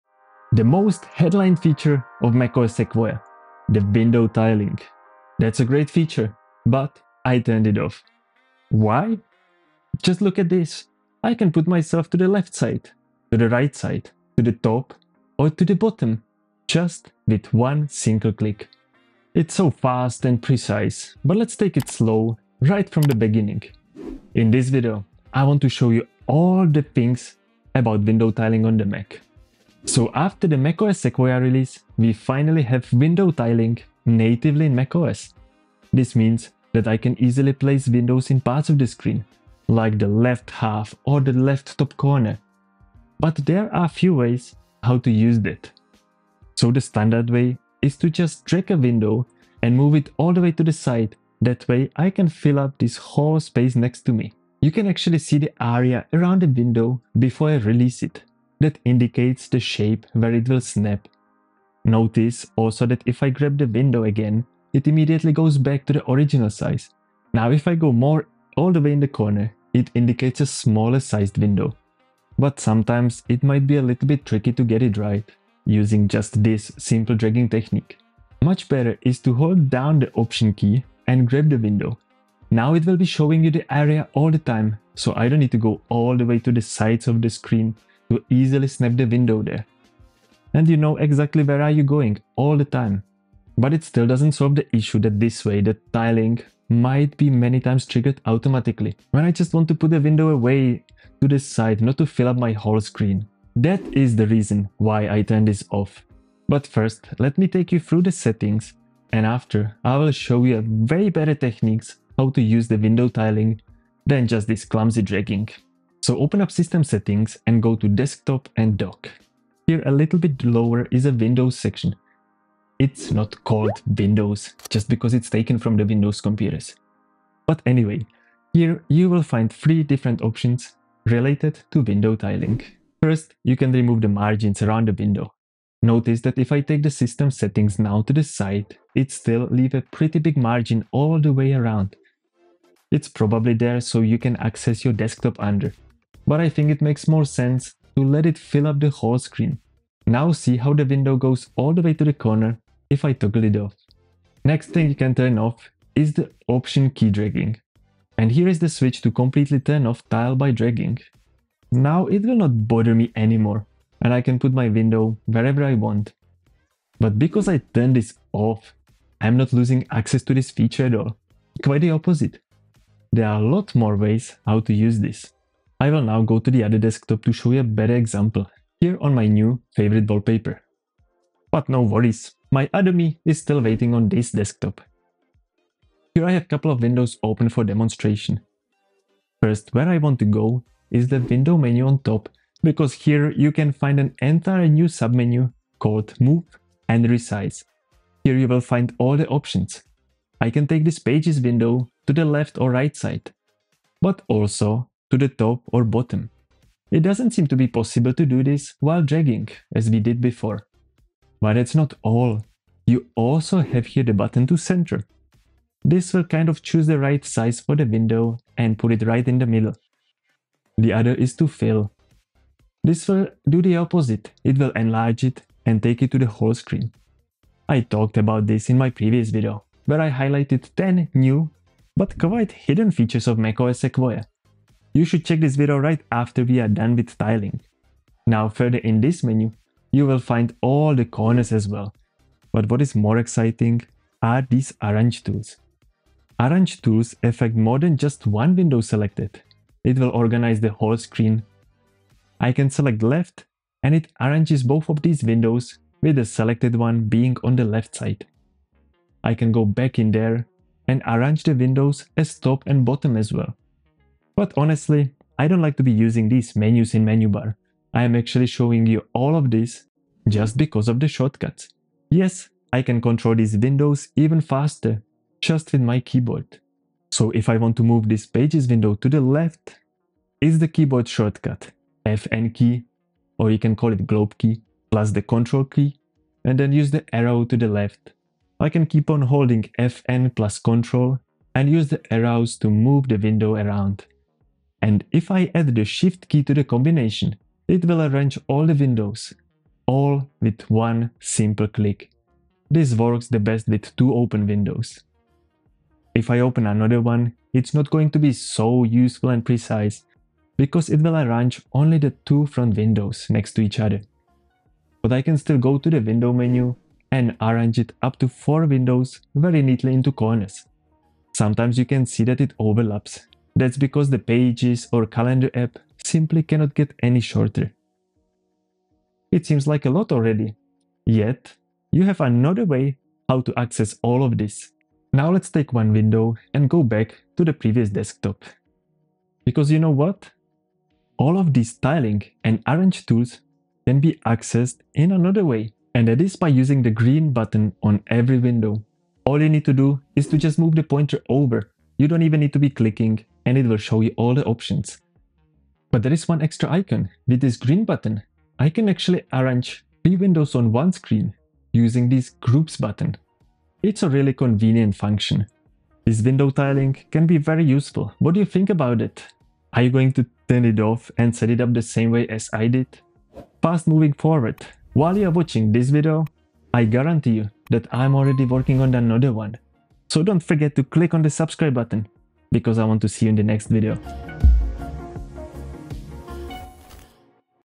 Window Tiling in macOS Sequoia - Why I Switched it OFF?
The most headlined feature of macOS Sequoia, Window Tiling is soo….
So good or so bad? I want to show you everything about window tiling on a Mac in one video

Do you want to get more than 200 apps for Mac on one place? Check out Setapp!

⏱ TIMESTAMPS
0:00 - Window Tiling in macOS Sequoia
0:29 - Tile by dragging windows
1:18 - Why I turned it off?
1:56 - All Window Tiling Settings
2:55 - Move & Resize Menu
3:58 - Arrange Tilled Windows
4:24 - Keyboard Shortcuts
5:34 - Green Button Options